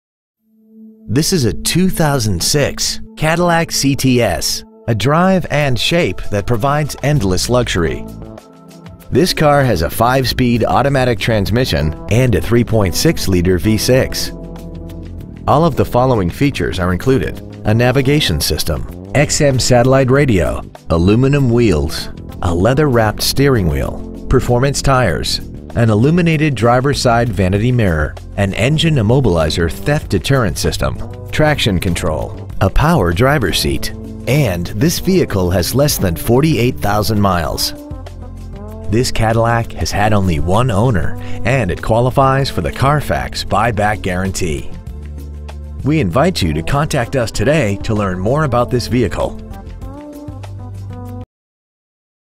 Preowned 2006 Cadillac CTS Merced CA 95340
We have been honored to serve the Merced CA area , we promise that your experience at our dealership will exceed your expectations !

 Year : 2006
 Make : Cadillac
 Model : CTS
 Engine : Gas V6 3.6L/217
 Trans . : Automatic
 Exterior : White Diamond
 Miles : 47,130
 Interior : Cashmere